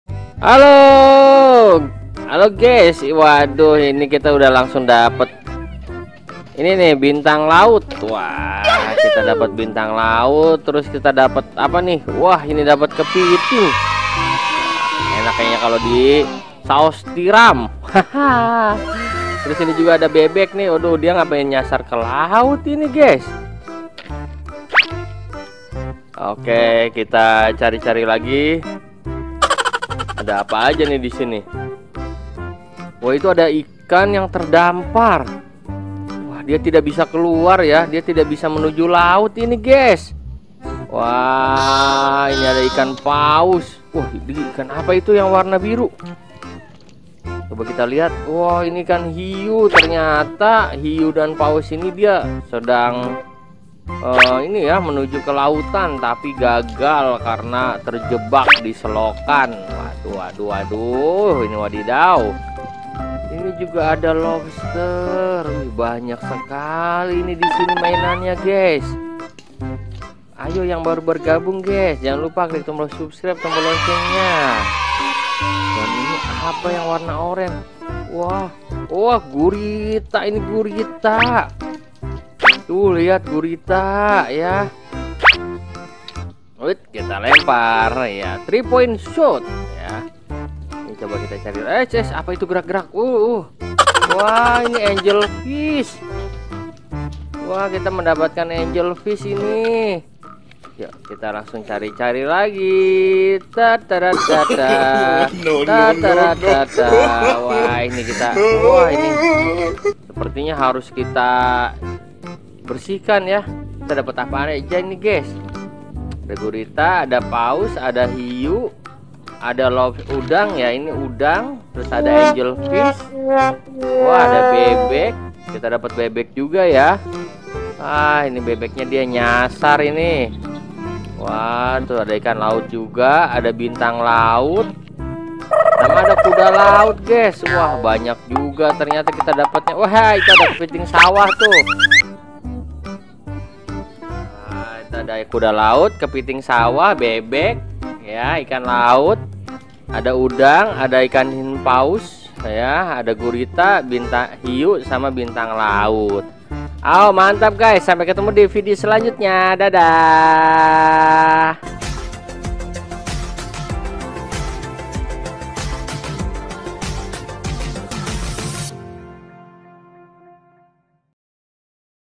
Mencari banyak Hewan Laut | Bintang Laut | Gurita | Ikan Hiu | Bebek | Lobster Besar
Keseruan kali ini berlanjut dengan Mencari banyak Mainan Hewan laut diSungai Kecil...